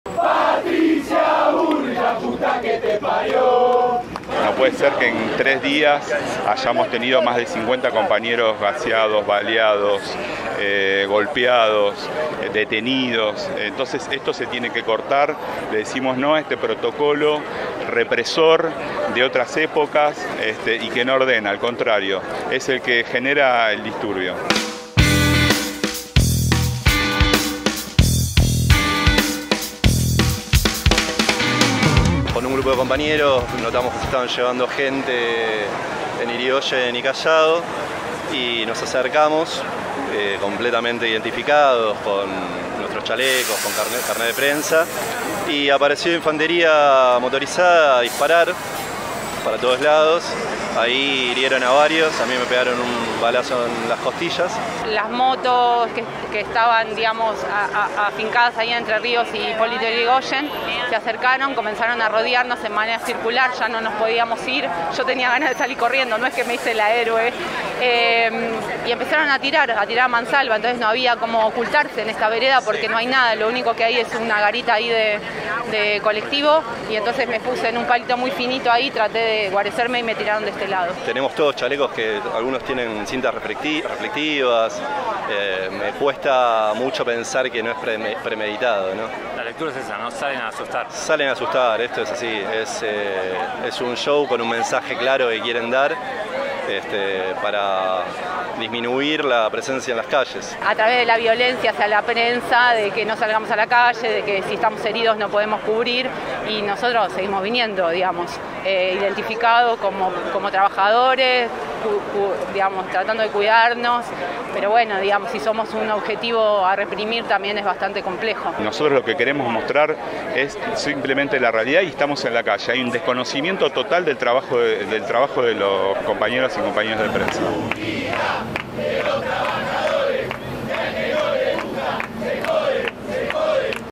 CAMARAZO contra la represión ordenada por la ministra Patricia Bullrich
Periodistas y fotógrafos repudian las agresiones de las fuerzas de seguridad  llevadas adelante durante los hechos de represión ilegal ocurridos el 1 y 2 de febrero en Plaza Congreso.